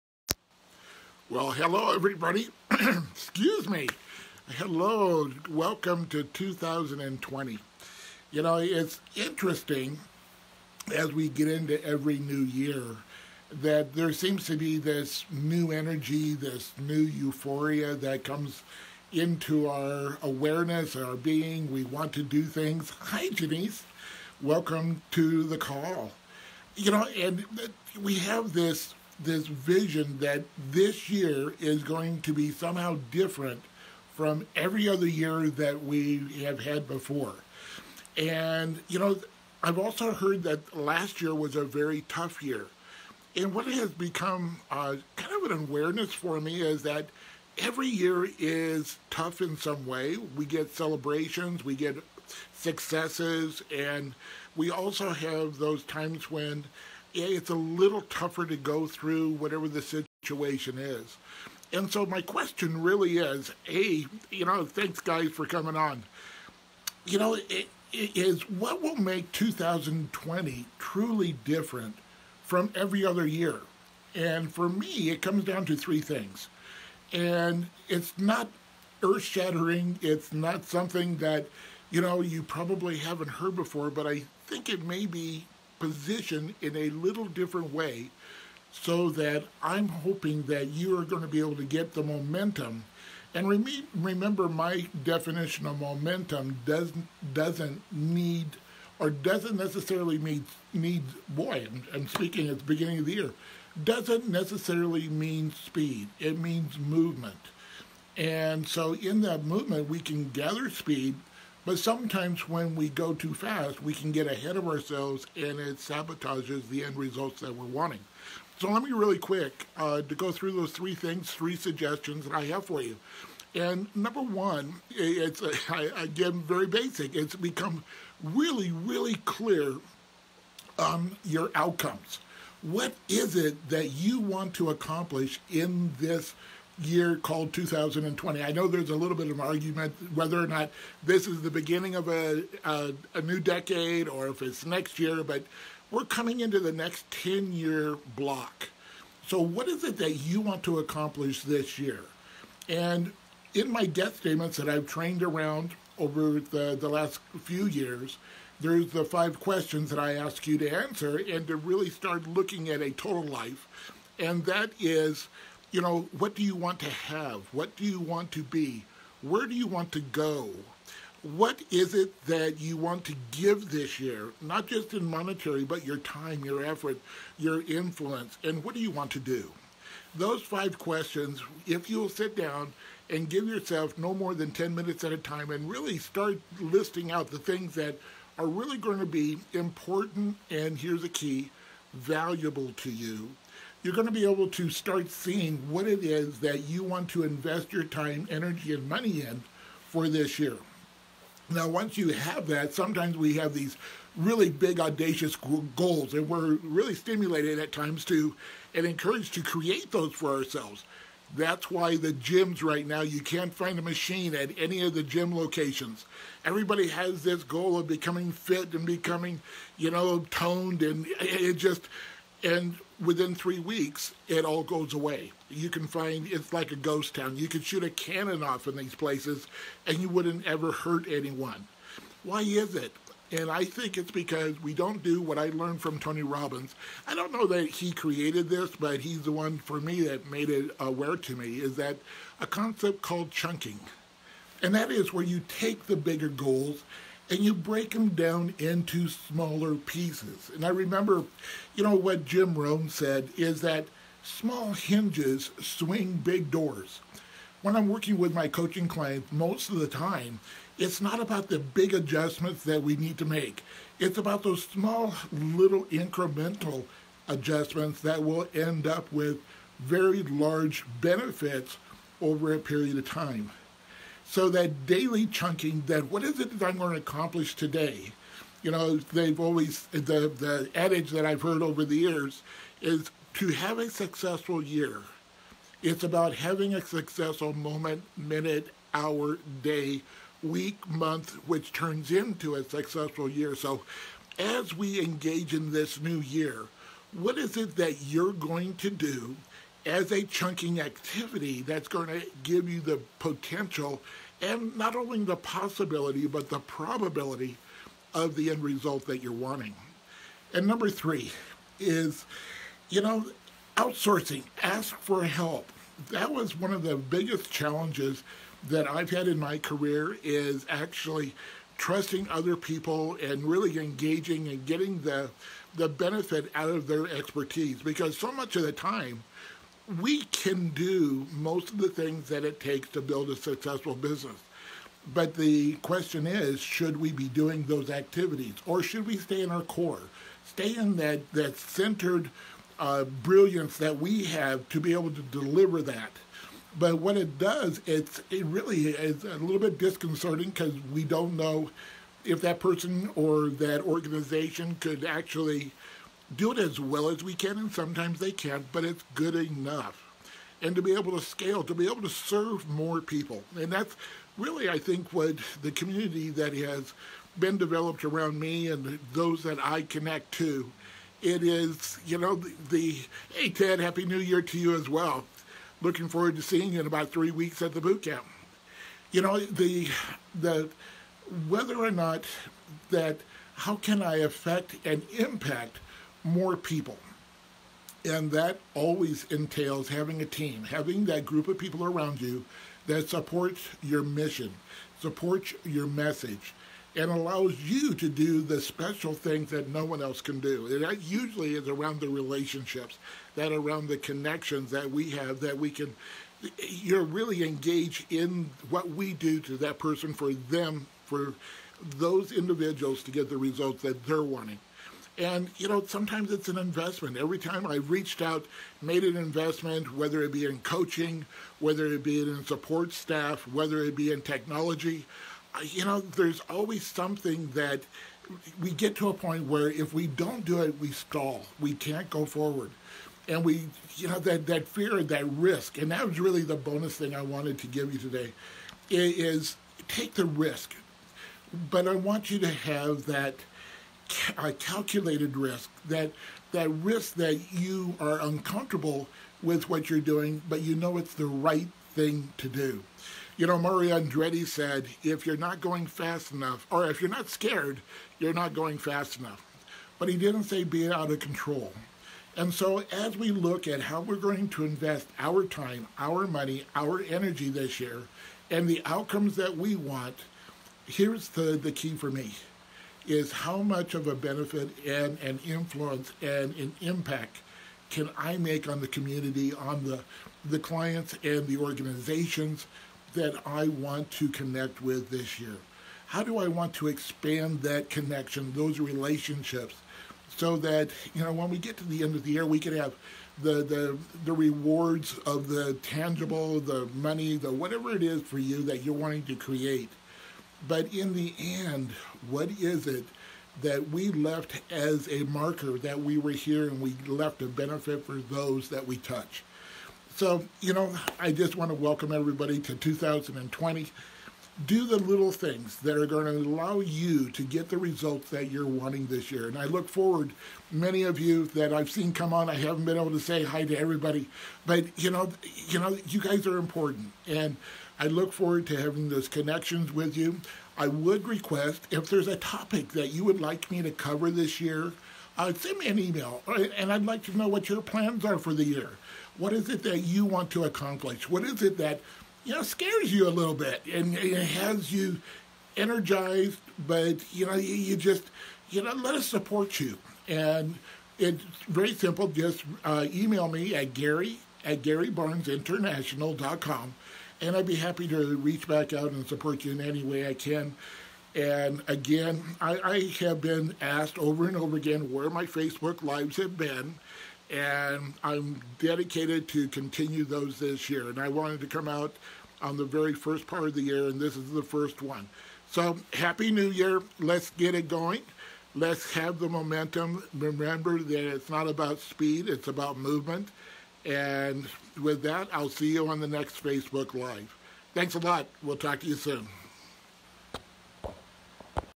What will make 2020 different from any other year
It seems that whenever we get to a new year, there seems to be a new energy or a new euphoria.  We have a vision that this year will somehow be different from every other year that we've had before.

So what will make 2020 truly different from the years before? For me, it comes down to 3 things.

1. Become really, really clear on your outcomes. What is it that you want to accomplish this year? Ask yourself these 5 questions - what do you want to have? Who do you want to be? Where do you want to go? What do you want to give this year? What do you want to do?  The answers to these will make it clear what you want to invest your time and money on this year.

2. Chunk it.  If you have a big audacious goal, chunk it down into smaller pieces instead of trying to do it all at once and then giving up in 3 weeks.  What will you do to chunk down your big goal and create a greater possibility of achieving them.

3. Ask for help. Stay in your centered brilliance core and hire others to do the rest. It's the way to scale your business and serve more people. Build a team that supports your vision and message.

In the end, what is it that we've left as a marker that we were here and left as a benefit for those we have touched?